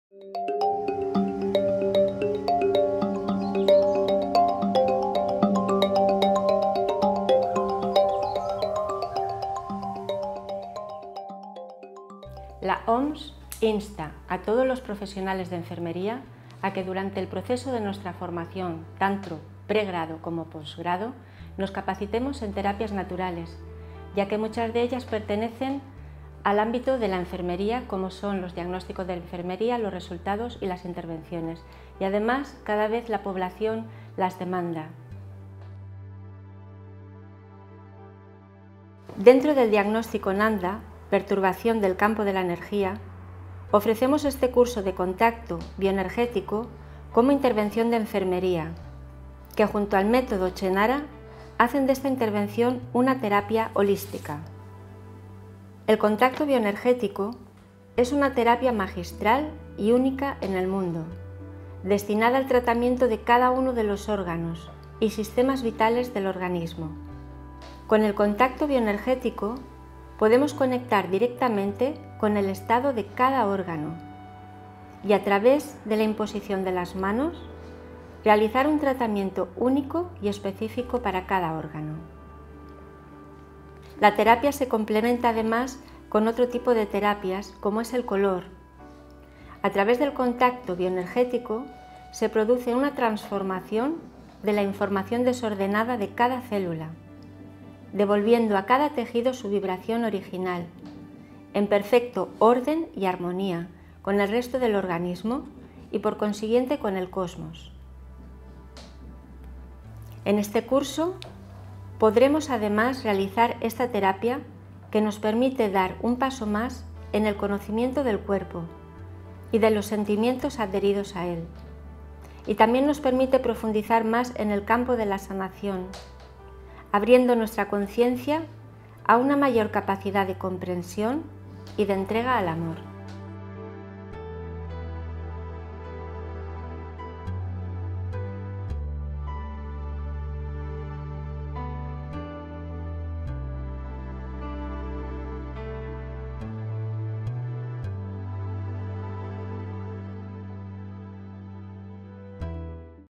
Contacto Bioenergético | Cecova TV
Con el contacto bioenergético se puede conectar directamente con el estado de cada órgano. A través de la imposición de las manos se puede realizar un tratamiento específico para cada uno de ellos.